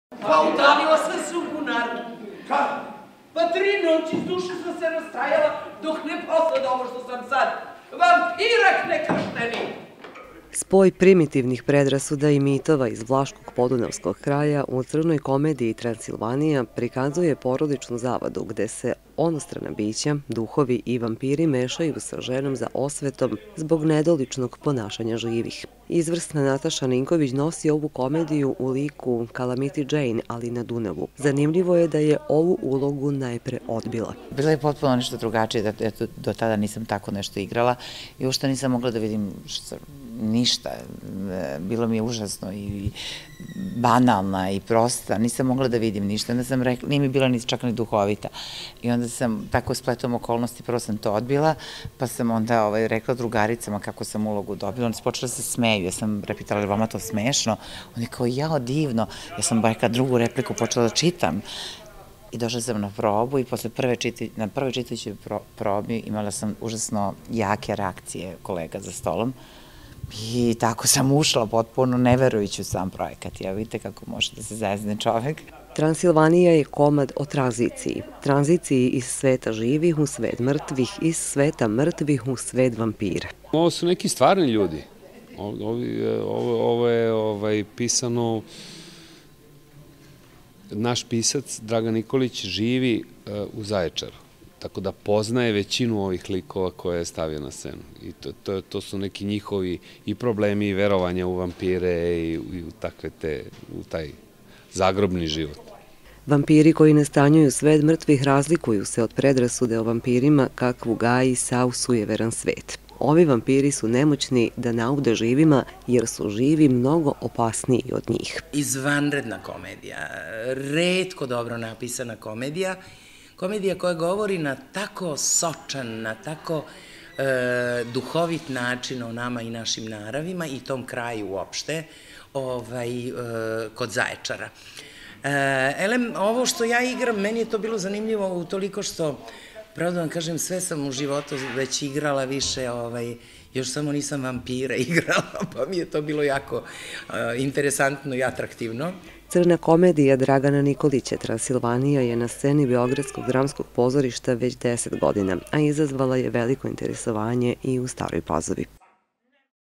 Predstava "Transilvanija"
Na pazovačkoj sceni  sinoć je izvedena predstava „Transilvanija“ Beogradskog dramskog pozorišta u režiji Milana Karadžića. Ova komedija, debitantski komad Dragana Nikolića, koja  sa podsmehom oslikava izvitopereno društvo u kome je sve tuđe prokleto, a sve naše ukleto  i koja već deset godina puni sale u Beogradu, izazvala je veliko interesovanje i u Staroj Pazovi.